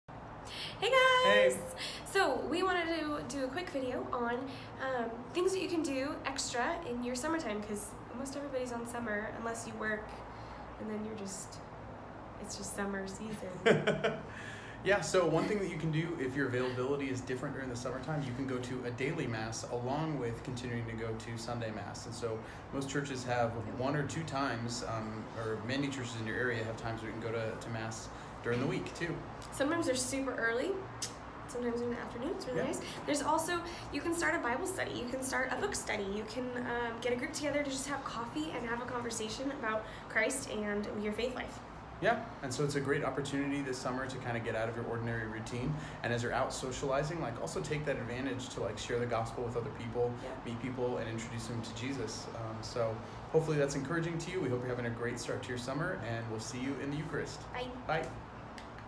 Summer Time!
Matt & Jenna talk about the opportunities we have to grow in our faith over summer now that some of your schedules may be a little more flexible!